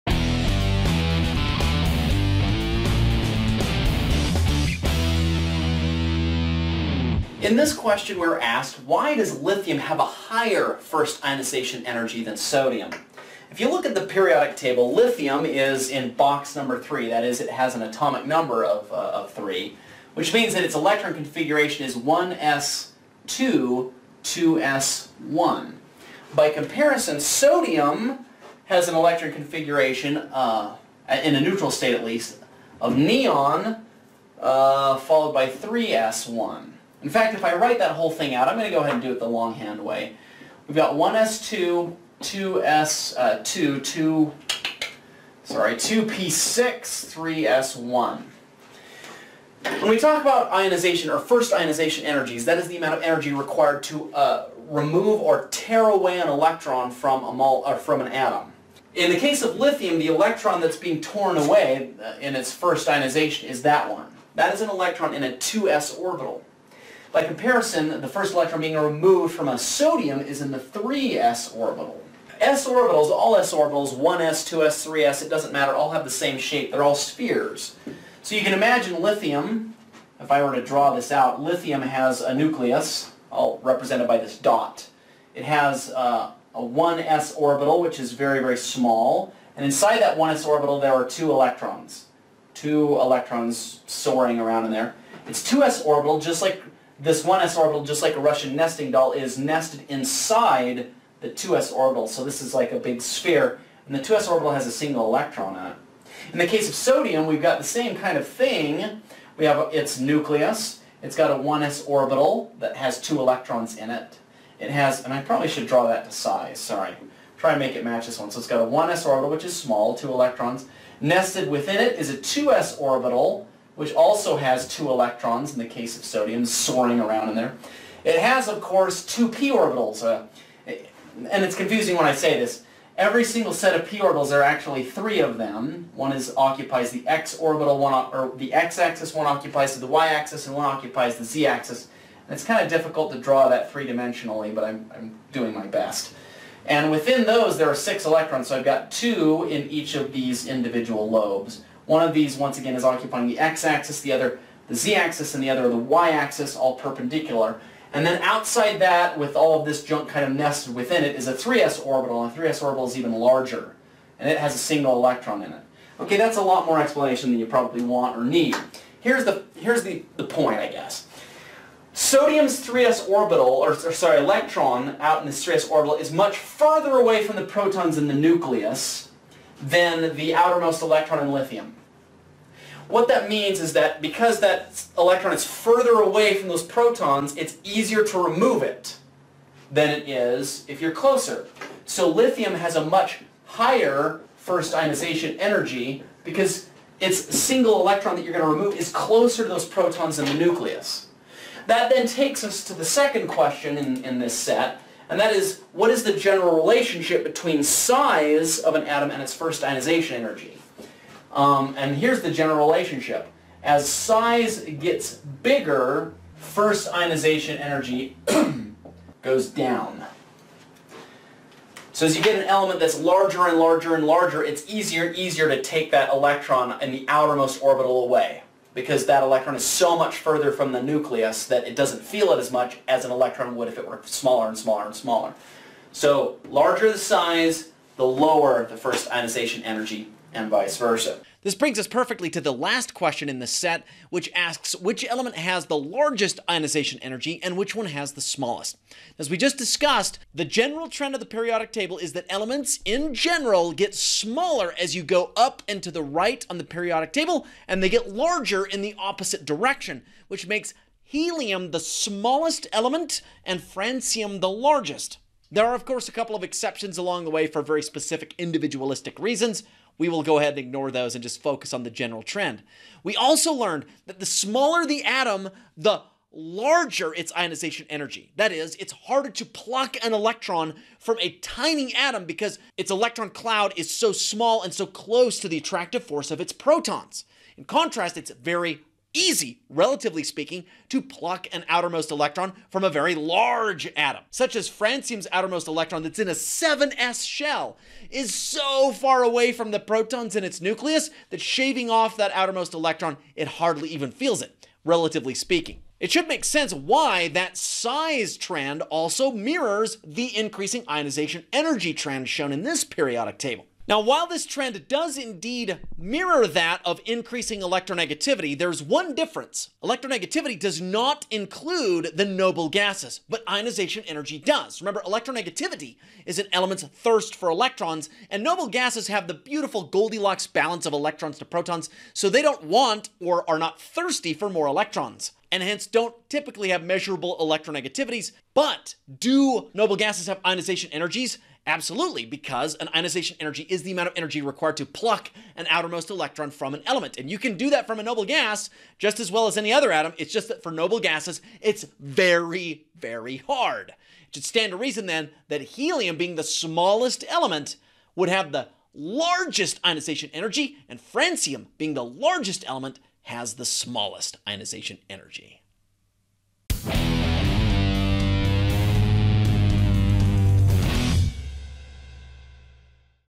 Chapter 7 – Part 11: Periodic Table Trends Sample Problem – Ionization Energy 1
In this video I’ll explain which element, Li or Na, has a larger first ionization energy. I’ll also explain the relationship between an element’s first ionization energy and its size.